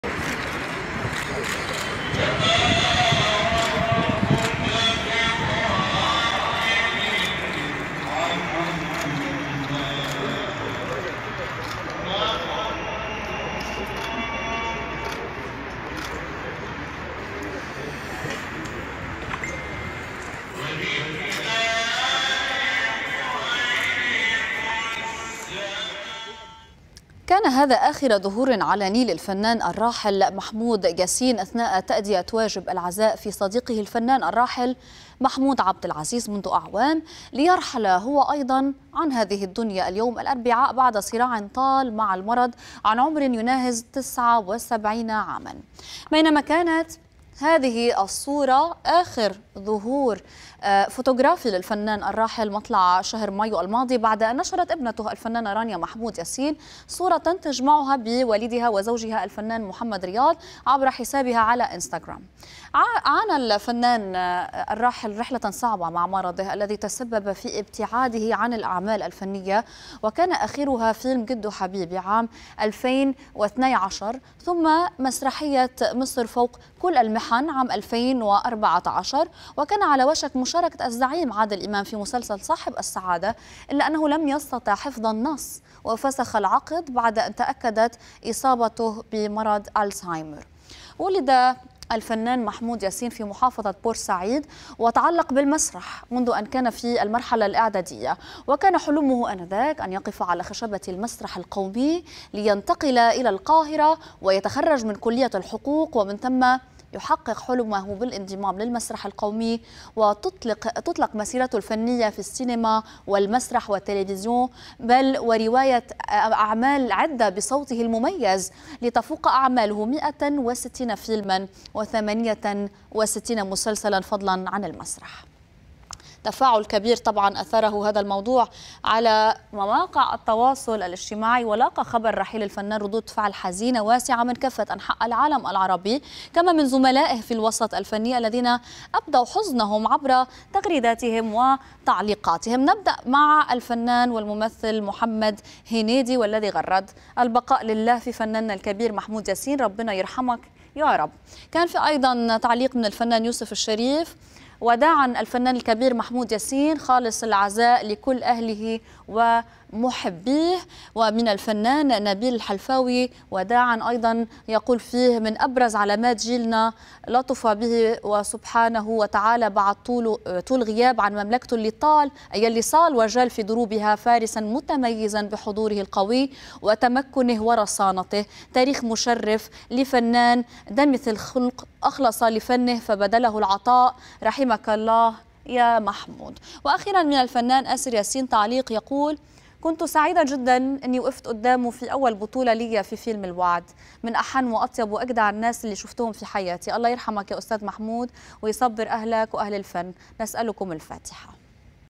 آخر ظهور للفنان محمود ياسين قبل وفاته
كان هذا آخر ظهور علني للفنان الراحل محمود ياسين أثناء تأدية واجب العزاء في صديقه الفنان الراحل محمود عبد العزيز منذ أعوام، ليرحل هو أيضا عن هذه الدنيا اليوم الأربعاء بعد صراع طال مع المرض عن عمر يناهز تسعة وسبعين عاما.